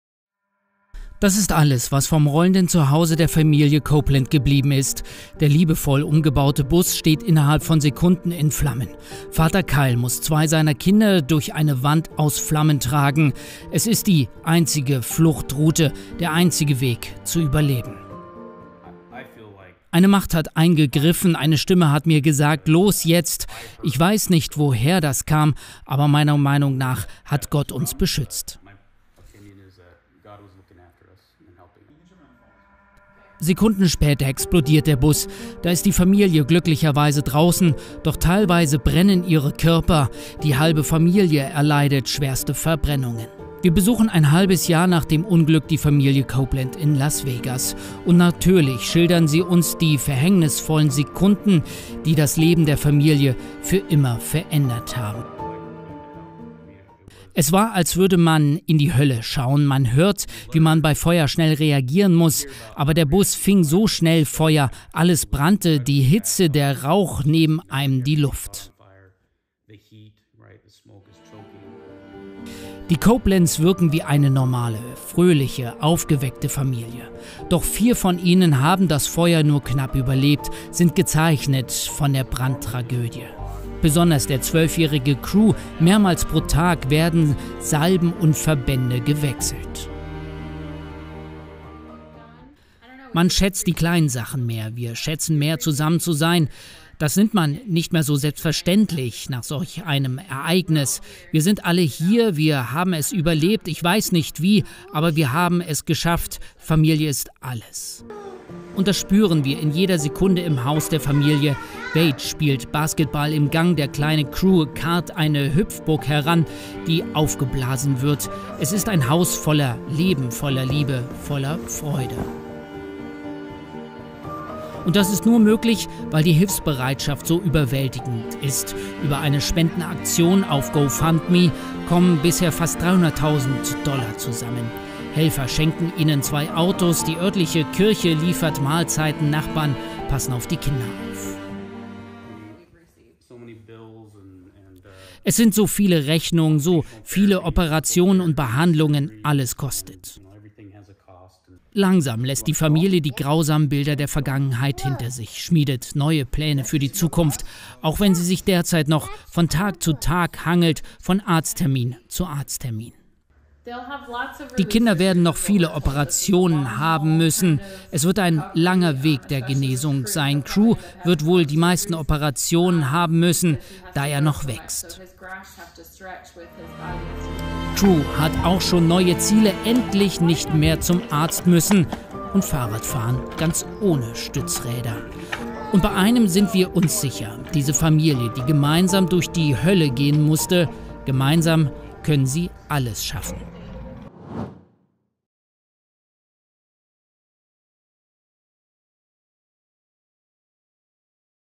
Kyle rettet seine Kinder vor dem Feuer-Tod | Las Vegas
Las Vegas (Nevada, USA) – Gefangen in einem brennenden Bus. Ihr einziger Fluchtweg führt durch eine Flammenwand. Vater Kyle Copeland (34) starrt in die Hölle. Dann schnappt er seine beiden Kinder und rennt los...

Die Überlebensgeschichte der Familie Copeland begann mit einem Traum. US-Veteran Kyle Copeland und seine Frau Whitney (33) kauften für 9500 Euro einen Bus, bauten ihn zu einer fahrenden Wohnung um – Boden aus Birkenholz, Kinderzimmer mit Stockbetten.

Die Familie wollte sich nach Papas Militärzeit wieder näher kommen. Also fuhren die Eltern mit ihren Kindern Krew, Emree, Kade und Pepper los, quer durch Amerika.

Bis der 27. Dezember 2021 ihr Leben für immer veränderte: Abends hörte Emree Geräusche aus dem Propan-Heizer, dann eine Explosion! Kyle: „Der Bus füllte sich mit Gas, der Brand brach überall aus!“